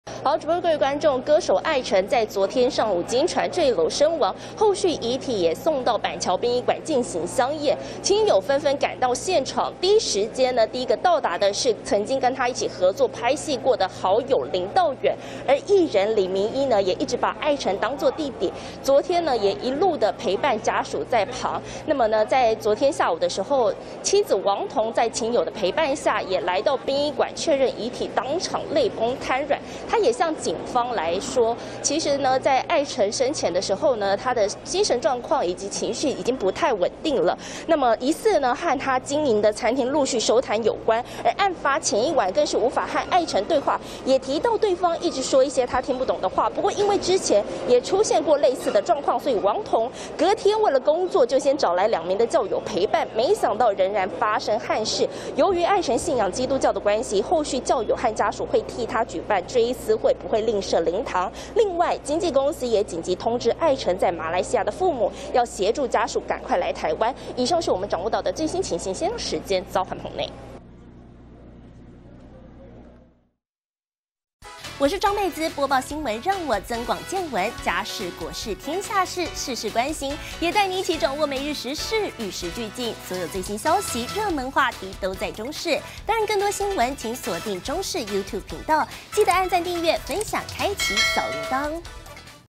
藝人艾成離世! 大馬家屬今抵台處理後事│中視新聞 20220818
歌手艾成昨天(17日)上午10點多在新北蘆洲住處墜樓，當場死亡，享年40歲，憾事發生後，妻子王瞳和親友也都到殯儀館處理後事，而目前最新情況連線記者張倍滋。